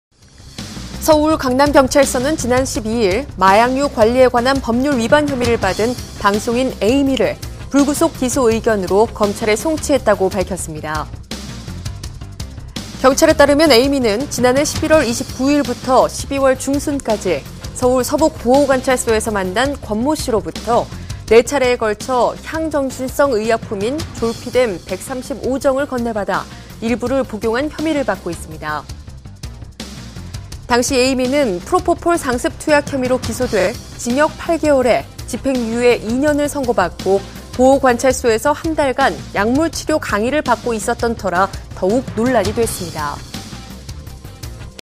[Y-STAR] Amy is called by police because of taking illegal pills (경찰, '졸피뎀 복용' 에이미 기소의견 송치)
서울 강남경찰서는 마약류관리에관한법률위반 혐의를 받은 방송인 에이미를 불구속 기소 의견으로 검찰에 송치했다고 지난 12일 밝혔습니다

경찰에 따르면 에이미는 지난해 11월 29일부터 12월 중순까지 서울 서부보호관찰소에서 만난 권모씨로부터 네 차례에 걸쳐 향정신성의약품인 졸피뎀 135정을 건네받아 일부를 복용한 혐의를 받고 있습니다

당시 에이미는 프로포폴 상습 투약 혐의로 기소돼 징역 8개월에 집행유예 2년을 선고받고 보호관찰소에서 한 달간 약물치료 강의를 받고 있었터라 더욱 논란이 됐습니다